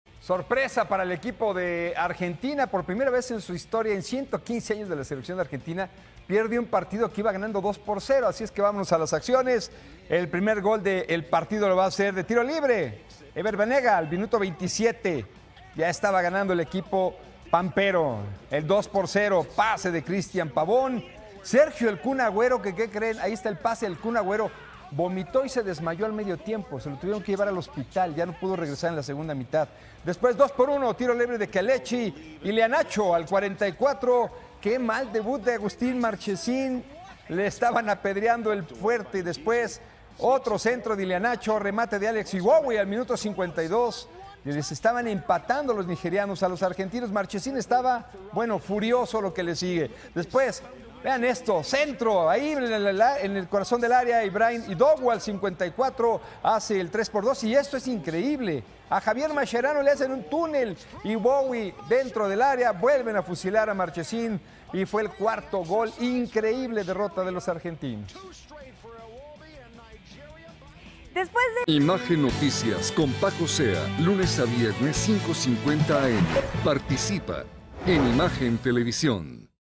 Primera vez que le pasa esto a Argentina | Noticias con Francisco Zea
El equipo de Argentina perdió un partido que iba ganando 2-0, esta es la primera vez que le ocurre esto en toda su historia.